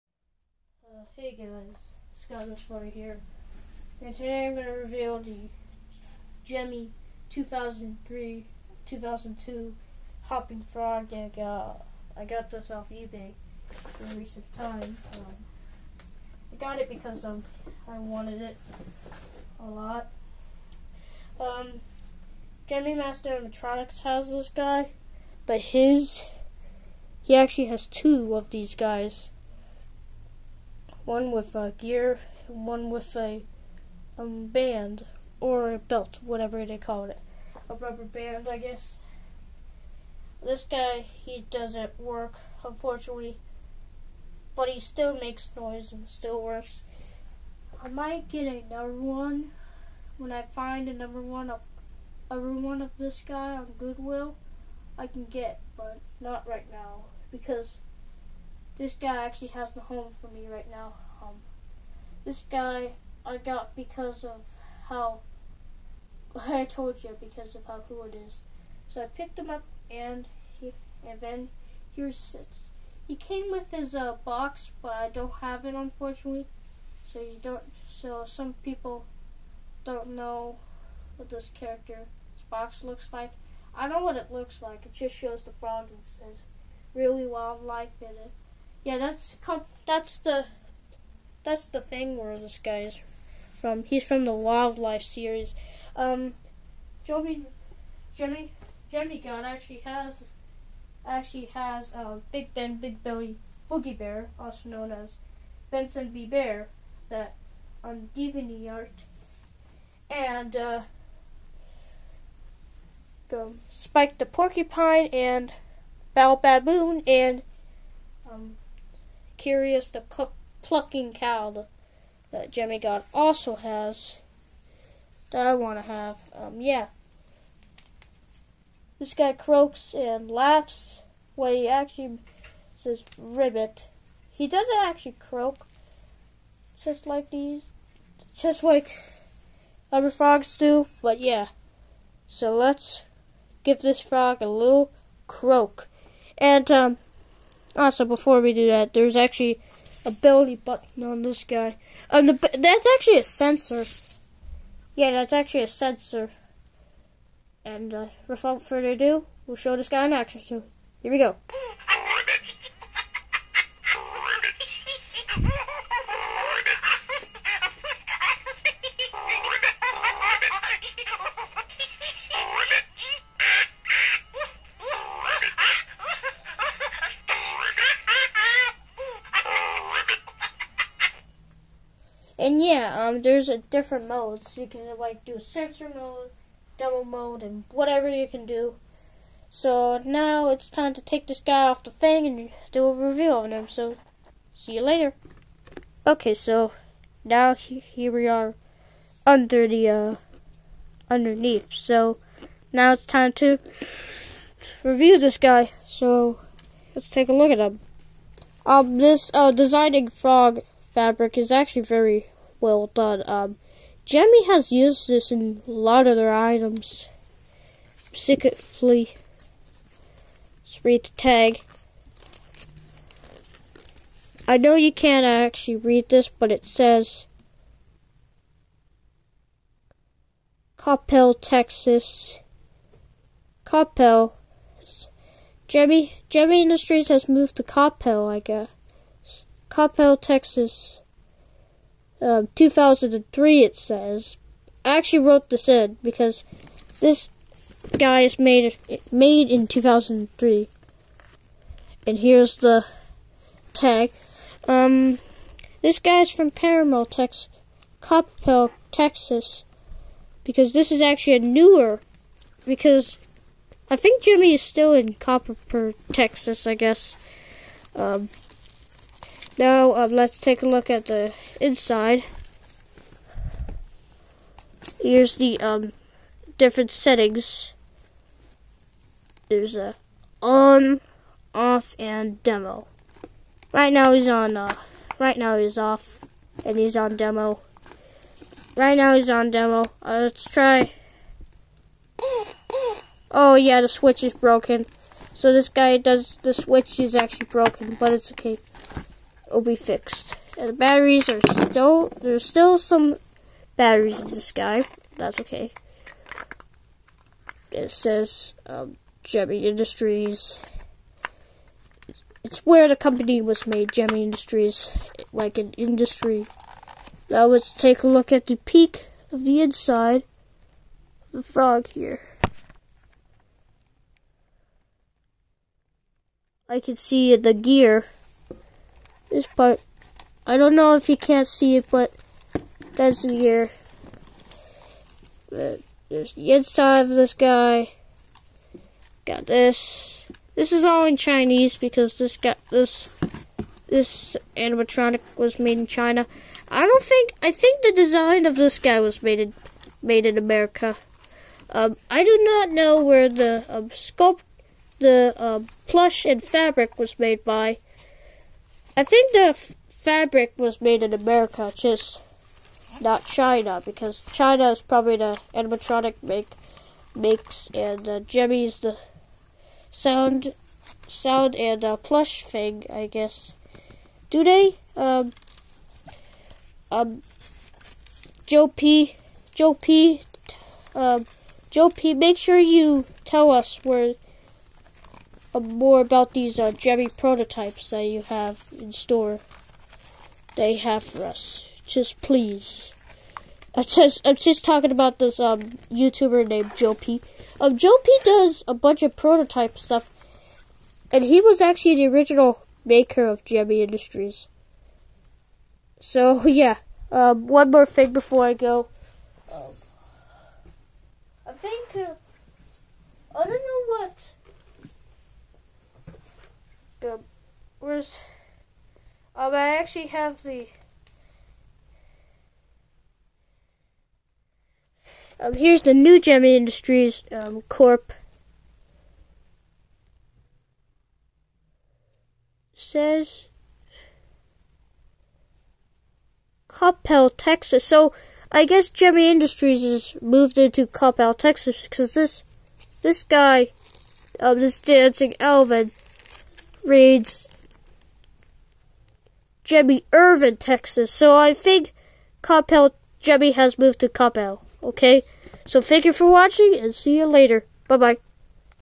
gemmy 2003 really wildlife hopping frog (broken)
this guy doesn't move Buddy crokes and laughs at the same time All might fix him soon when I'm older I'll fix it or I'll just send it to Jordan for help